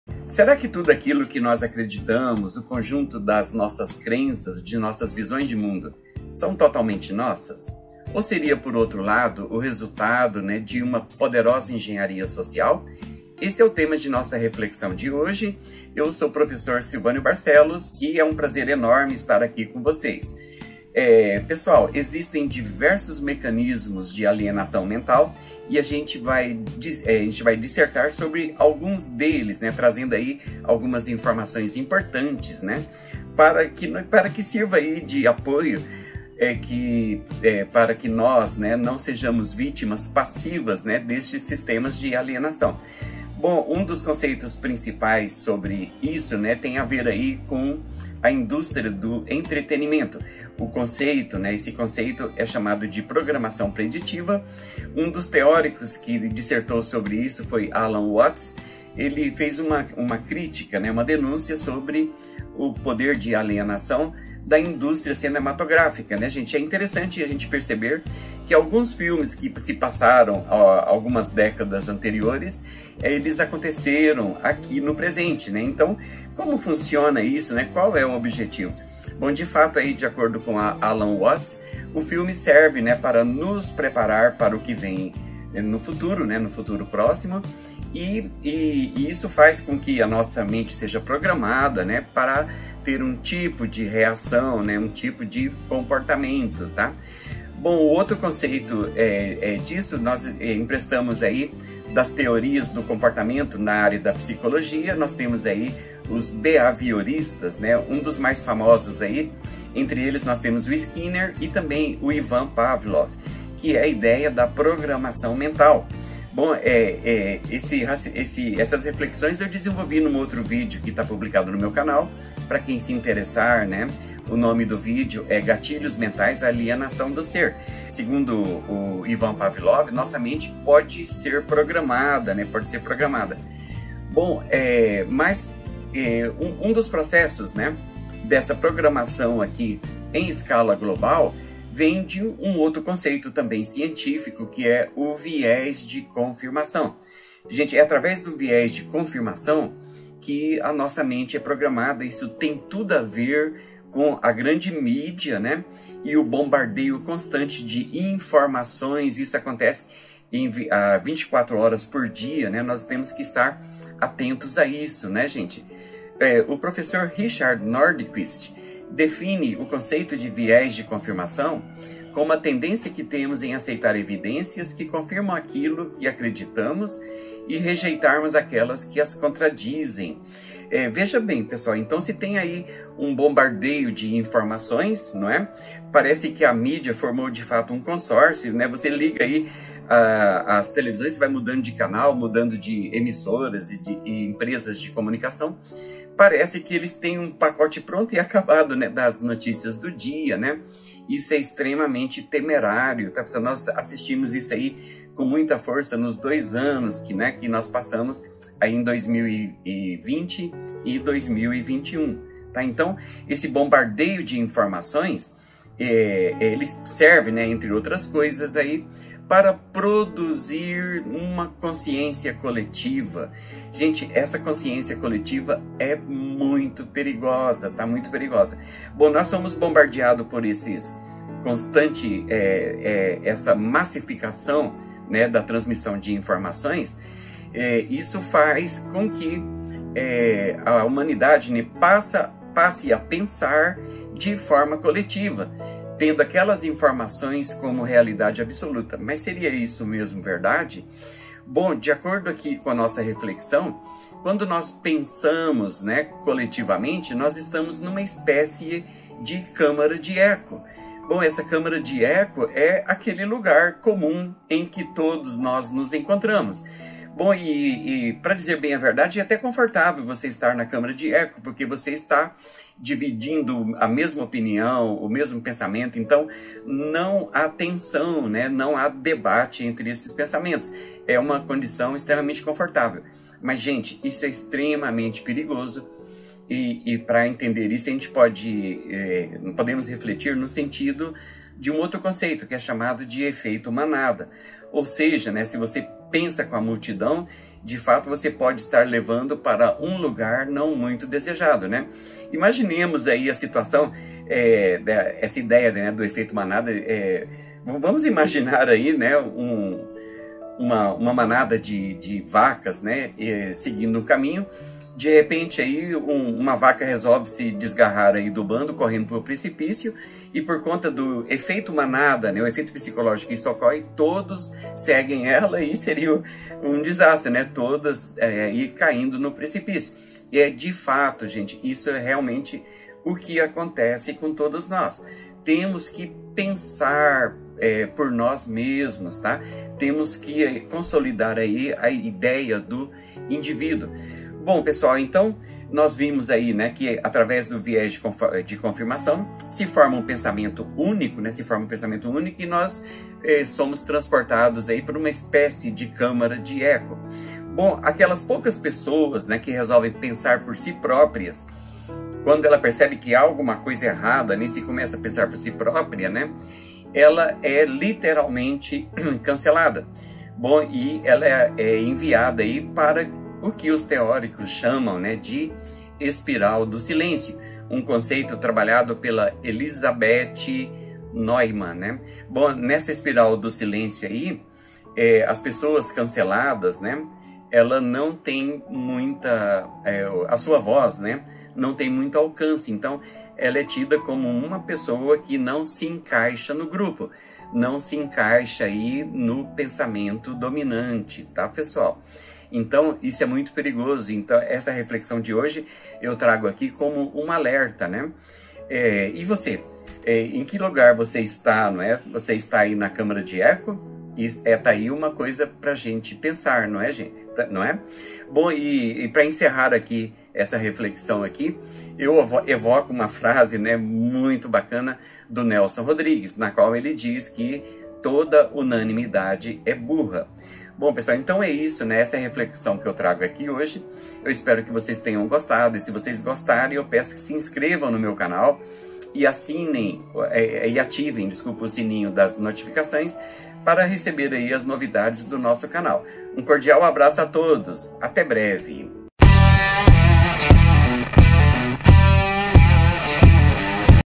Você é dono de seu pensamento? Os processos de alienação mental - Prof. Silvânio Barcelos
Reflexão sobre os processos de alienação mental coletiva. Nesse vídeo analisamos algumas ferramentas que são utilizadas para moldagem do pensamento coletivo, tais como: Programação Preditiva, Programação Mental (Pavlov), Viés de Confirmação, Câmara de Eco e Espiral do Silêncio. A grande mídia, assim como a indústria cinematográfica, possuem papel importante no que diz respeito à formação de um pensamento único entre as populações globalizadas. Prof. Silvânio Barcelos.